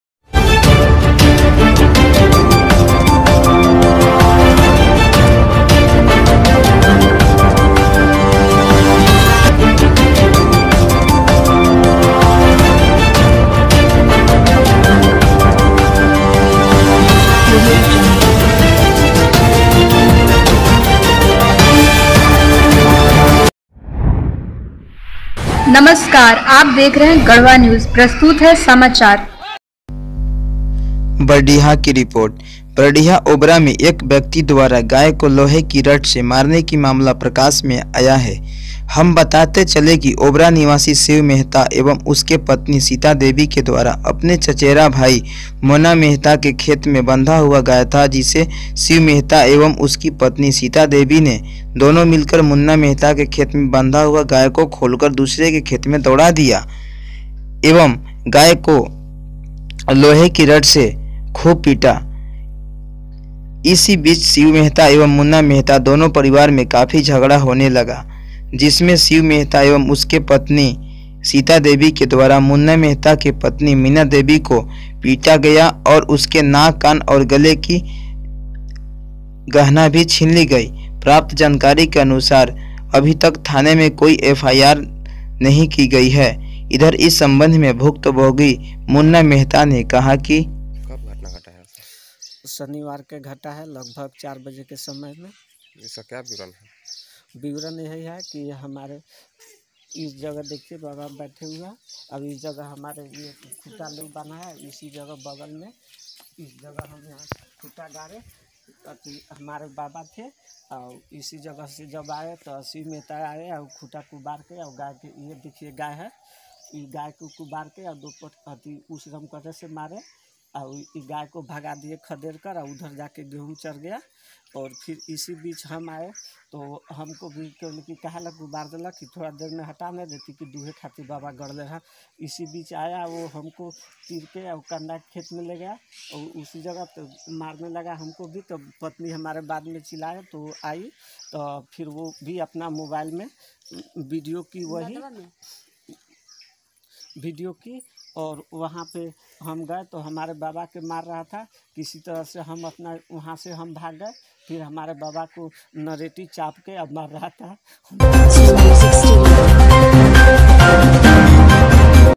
एक व्यक्ति ने गाय को लोहे की रॉड से खूब पीटा// Ss Garhwa news tv
गढ़वा न्यूज़, बरडीहा: ओबरा में एक व्यक्ति द्वारा गाय को लोहे की रॉड से मारने की मामला प्रकाश में आया है l

हम बताते चलें कि ओबरा निवासी शिव मेहता एवं उसके पत्नी सीता देवी के द्वारा अपने चचेरा भाई मोना मेहता के खेत में बंधा हुआ गाय था जिसे शिव मेहता एवं उसकी पत्नी सीता देवी ने दोनों मिलकर मुन्ना मेहता के खेत में बांधा हुआ गाय को खोलकर दूसरे के खेत में दौड़ा दिया गया एवं लोहे की रॉड से गाय को खूब पीटा गया इसी बीच शिव मेहता एवं मुन्ना मेहता दोनों परिवार में काफी झगड़ा होने लगा जिसमें शिव मेहता एवं उसके पत्नी सीता देवी के द्वारा मुन्ना मेहता के पत्नी मीना देवी को भी पीटा गया और उसके नाक कान और गले की गाना भी छीन ली गई प्राप्त जानकारी के अनुसार अभी तक थाने में कोई f.i.r. नहीं की गई है l